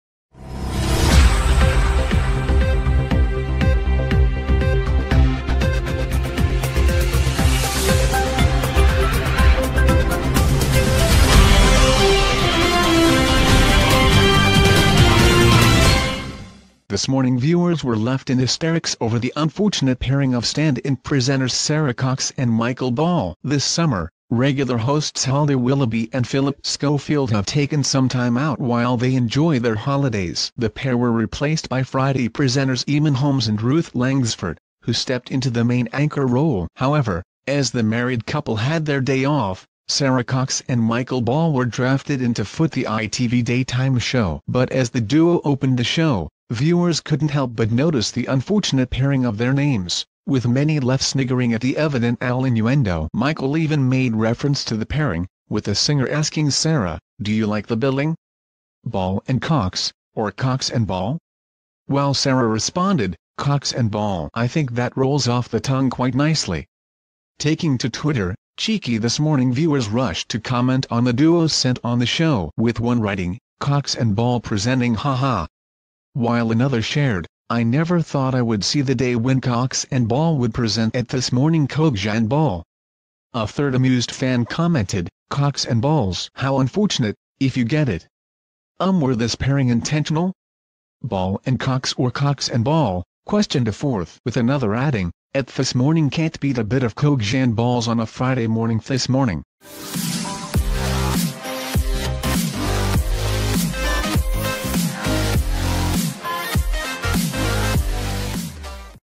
This Morning fans in HYSTERICS at unfortunate innuendo as Sara Cox and Michael Ball step in as
This Morning fans in HYSTERICS at unfortunate innuendo as Sara Cox and Michael Ball step in as presenters
This Morning viewers were left in hysterics over the unfortunate pairing of stand-in presenters Sara Cox and Michael Ball.
This summer, regular hosts Holly Willoughby and Phillip Schofield have taken